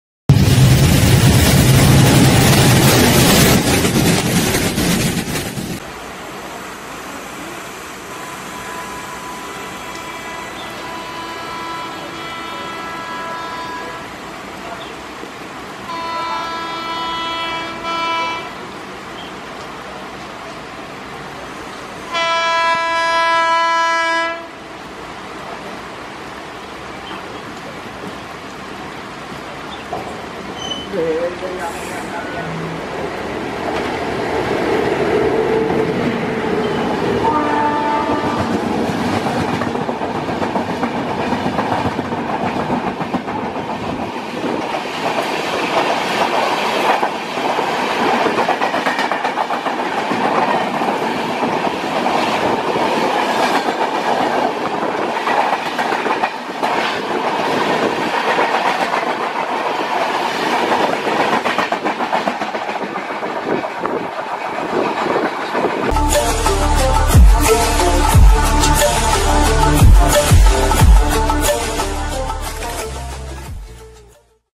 Nonstop Heavy Rain: Beautiful Konkan Railways:Janshatabdi Express VS Mangala Lakshadweep Express
12617 Ernakulam NZM Mangala Lakshadweep Express Sandwich Crossing 12484 Amritsar Kochuveli Exp & 12051 Mumbai Madgaon Janshatabdi Express At Sangameshwar Road,Konkan Railway